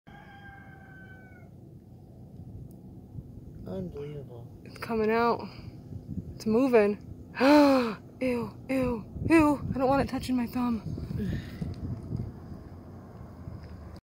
Botfly bot fly larva coming out of a dead rat. Most horrifyingly disgusting thing you'll ever see
Poor little rat drowned in the water bowl pull them out and was shocked and utterly disgusted by what I saw. I thought it was an abscess until I looked closer and it was a larva bigger than my pinky toe pushing its way out of the dead body of a drowned rat.